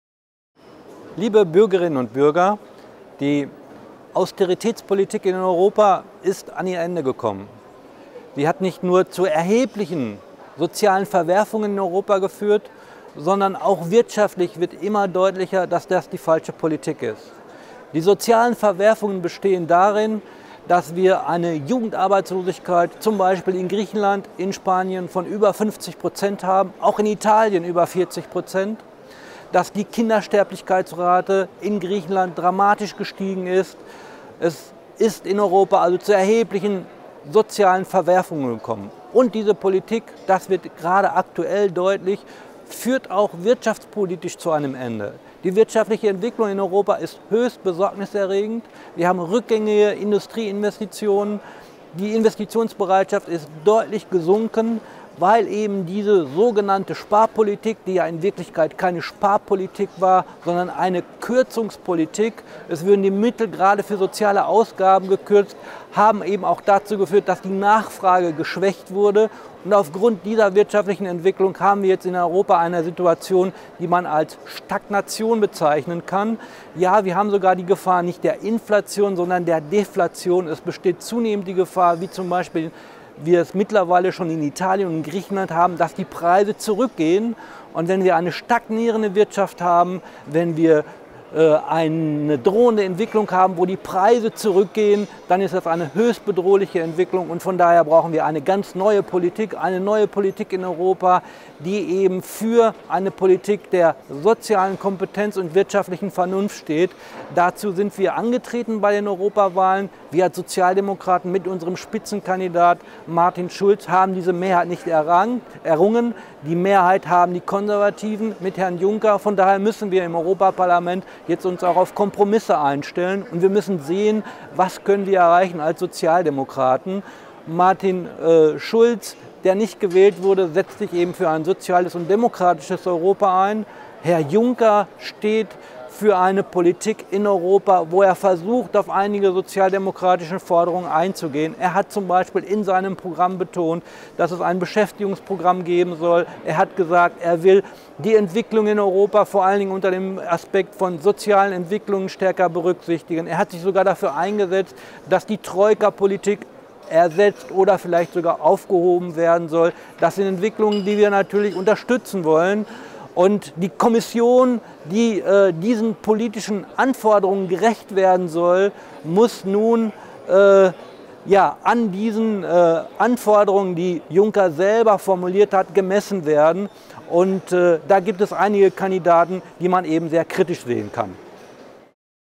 Dr. Dietmar Köster, MEP zur Austeritätspolitik der EU
Die Austeritätspolitik der europäischen Nationalstaaten, welche die Europäische Union in den letzten Jahren im Wesentlichen geprägt hat, ist gescheitert! Die Rückläufige Entwicklung der Wirtschaft zeigt dies eindeutig auf. Neben diesem Faktor sind die humanen Folgen jedoch mittlerweile offen sichtbar. So hat beispielsweise der Spardruck in Griechenland dazu geführt, dass die Kindersterblichkeit deutlich gestiegen ist. Das Ziel nachhaltiger Wirtschaftspolitik sollte nicht das Vermeiden von Staatsschulden um jeden Preis sein, sondern das gezielte Setzen von Wachstumsimpulsen. Der Europaabgeordnete Dr. Dietmar Köster erklärt in diesem Video, warum die Politik auf der Europäischen Ebene, welche im Wesentlichen von den Konservativen geprägt wird, endlich durch investigative Maßnahmen Wachstumsimpulse setzen sollte.